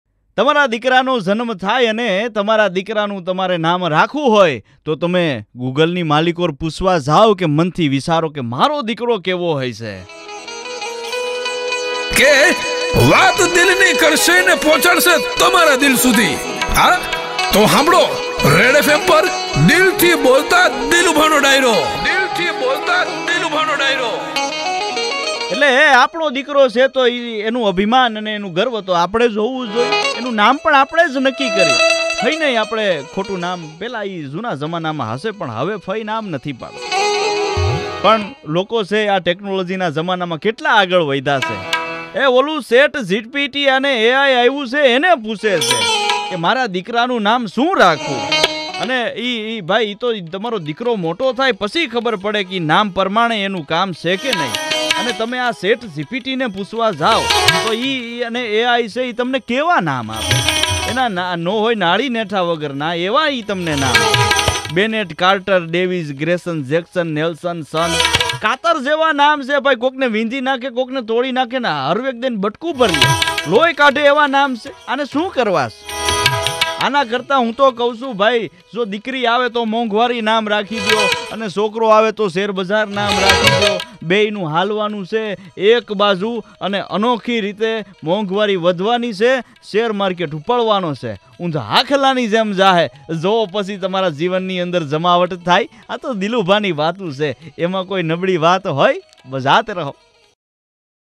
છોકરાવનું નામ આમ રખાય..😂 | Dilubha | RJ Jay | Jay Sakariya | Dayro | comedy vlog | Daily Vlog | fun
આ દિલુભા દિલથી બોલે અને થોડું અઘરું લાગે, કારણકે એમાં એવું છે કે, દિલુભાને બધા કરતા કૈક અલગ જ વિચાર આવે, એટલે સામે વાળાને થોડી તકલીફ પડે. પણ ચિંતા નહિ કરતા એમાં કંઈક સારી વાત છુપાયેલી હોય છે. હવે સાંભળી જ રહ્યા છો તમે... છે ને જમાવટ..